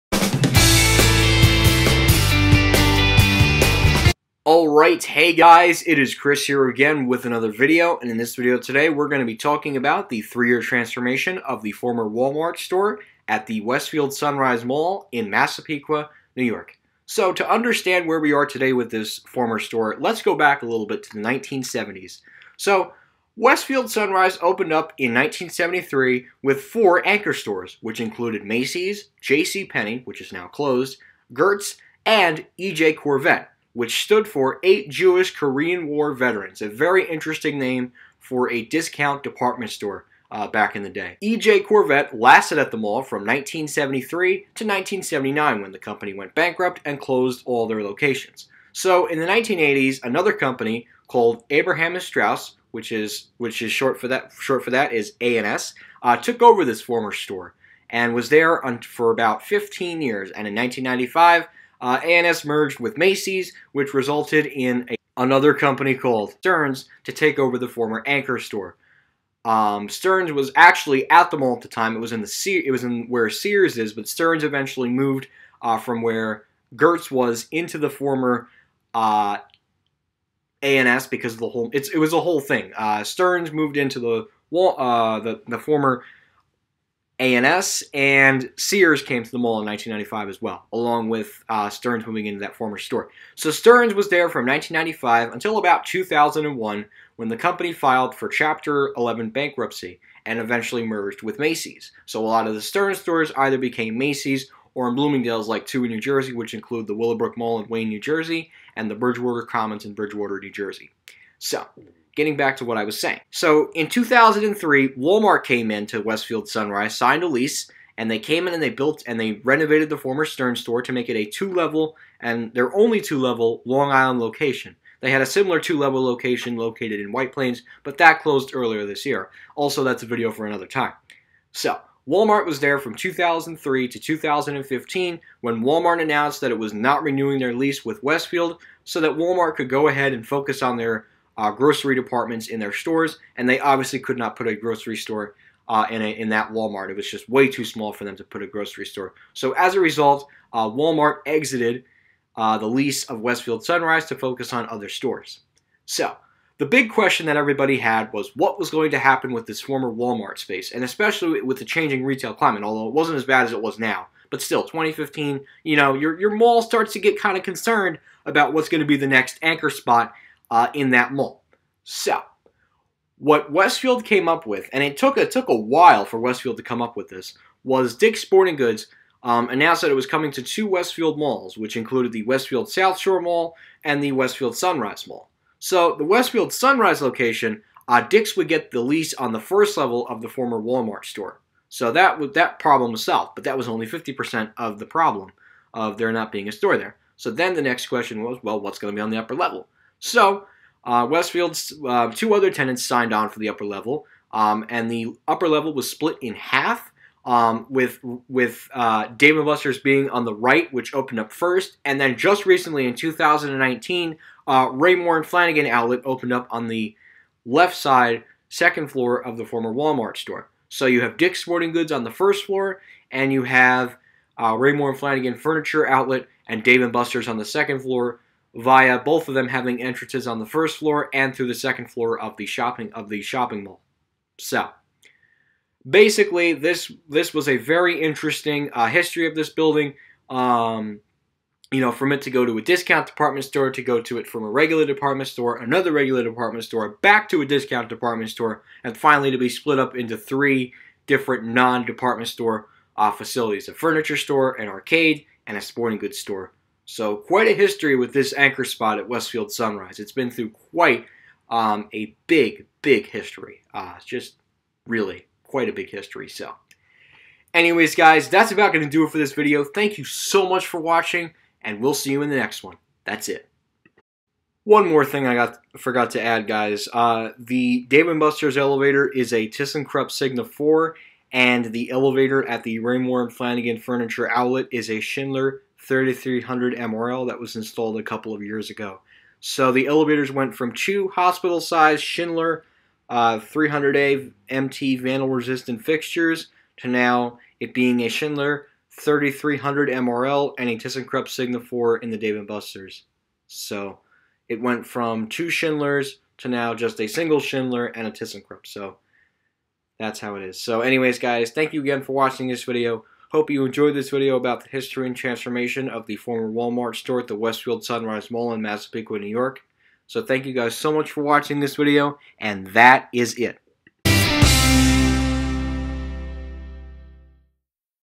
Transformation and History of the Former Walmart Space at Westfield Sunrise in Massapequa, New York
This is a brief History of this department store at the Westfield Sunrise as it was known as many different things and is now several smaller store instead of being a big box department Store.